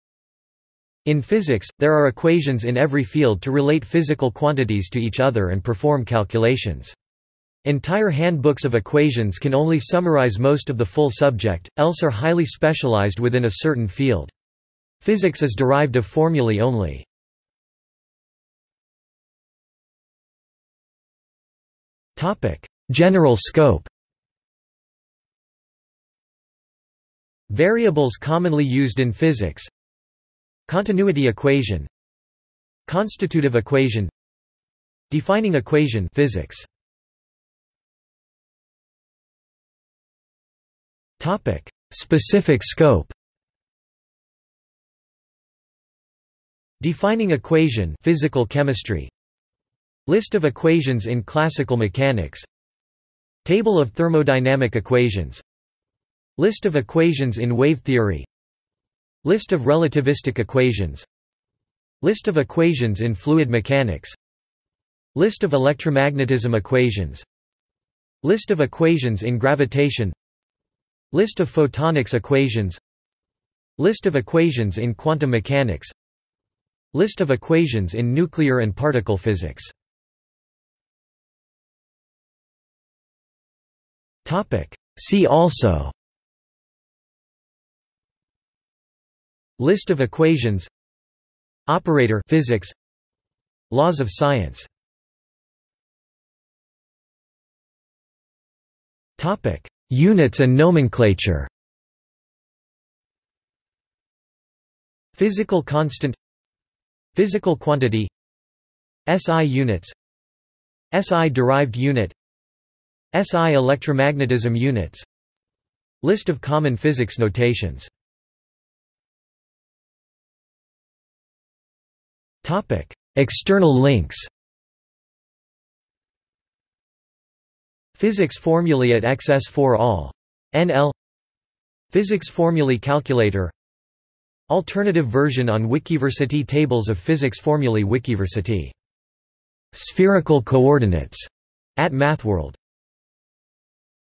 Elementary physics formulae | Wikipedia audio article
00:00:18 1 General scope
00:00:37 2 Specific scope
00:01:20 3 See also
00:01:35 4 Units and nomenclature
00:01:58 5 External links

- Socrates

SUMMARY
In physics, there are equations in every field to relate physical quantities to each other and perform calculations. Entire handbooks of equations can only summarize most of the full subject, else are highly specialized within a certain field. Physics is derived of formulae only.